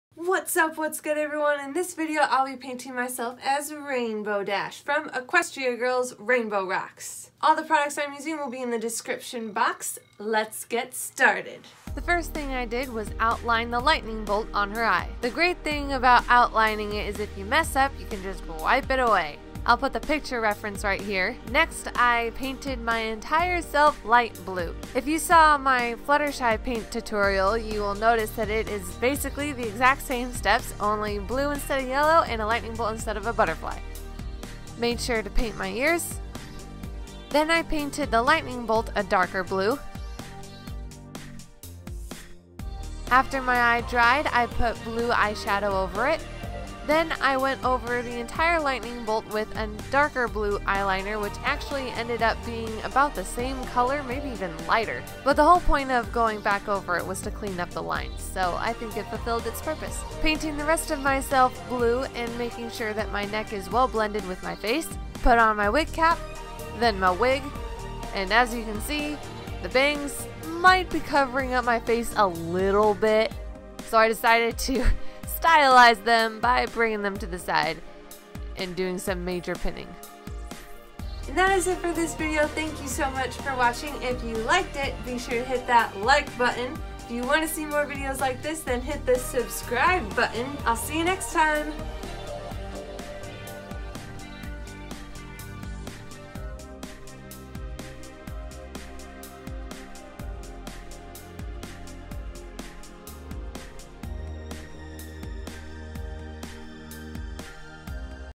Rainbow Dash Equestria Girl Makeup
Face painting Rainbow Dash from My Little Pony Equestria Girls.